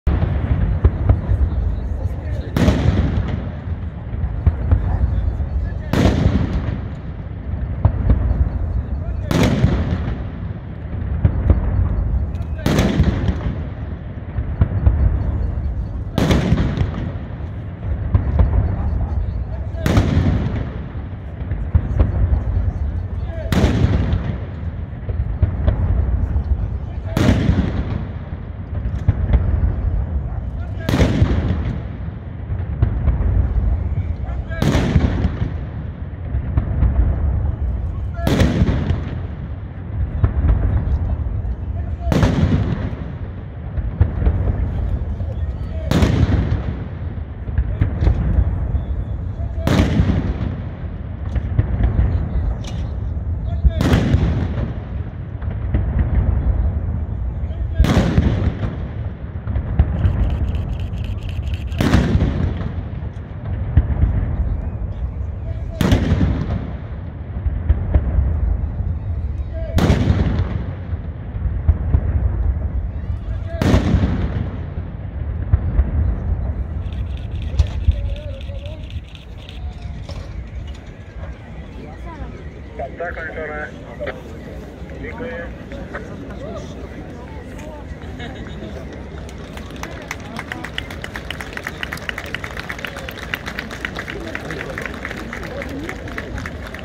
У Польщі відзначають День конституції
У Варшаві відбуваються урочистості з нагоди Дня конституції. Президент Польщі Анджей Дуда привітав поляків з цією датою та вручив на Королівському замку державні нагороди.
О 12-й годині на площі Юзефа Пілсудського відбувся салют з нагоди свята.